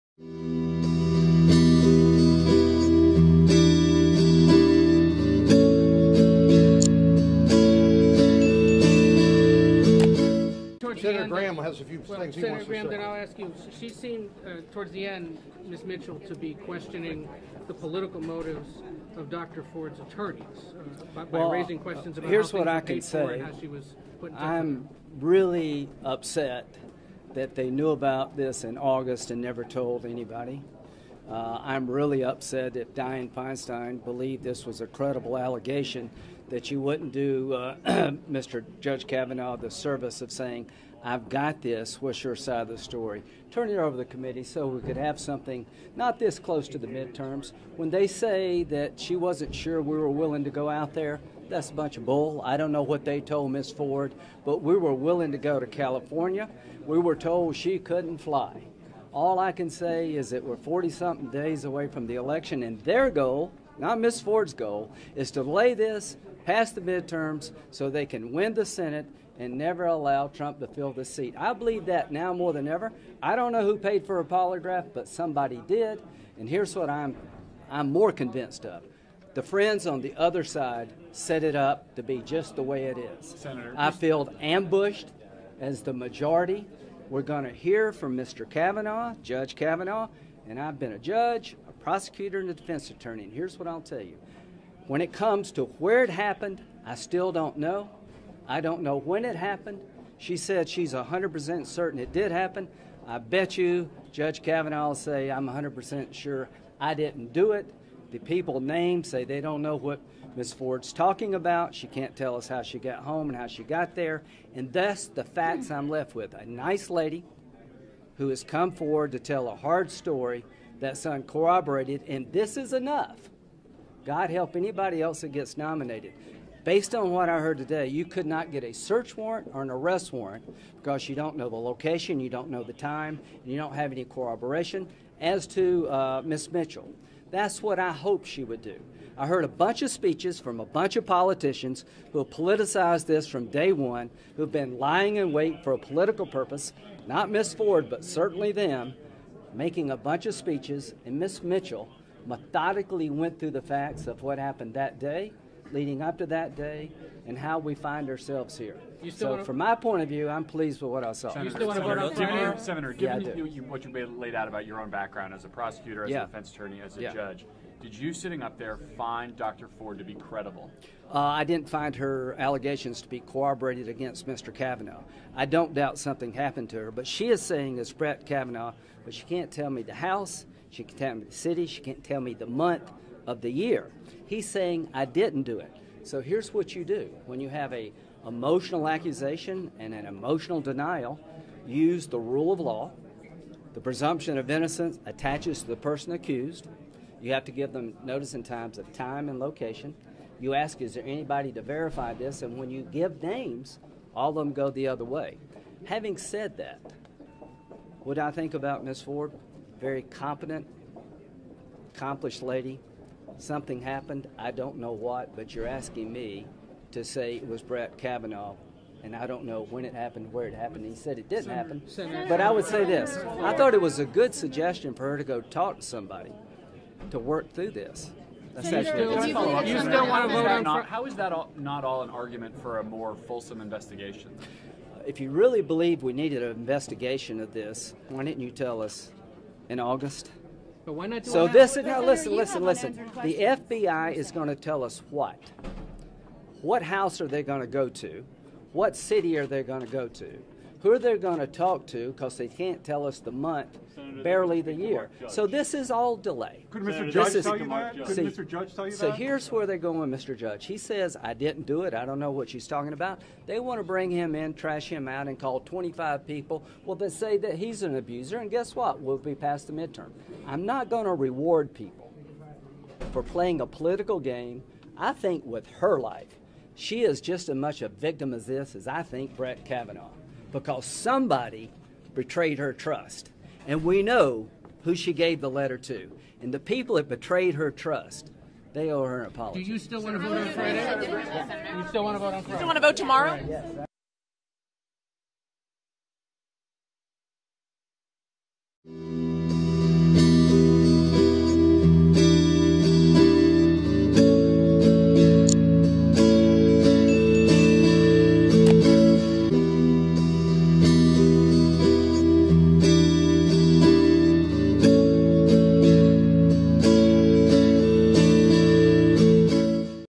Lindsey Graham Talks With Reporters After Christine Blasey Ford's Testimony Before Senate
Lindsey Graham Talks With Reporters After Christine Blasey Ford's Testimony Before Senate Judicial Committee
Lindsey Graham Talks With Reporters After Christine Blasey Ford'
s Testimony Before Senate Judicial CommiMCMP-001049 Mystic Chords of Memory Project Video Archive Historian's Eye
Was Ford Super Girl? Judge Kavanaugh was a athlete, she had the strength to keep from getting raped by Kavanaugh and his buddy?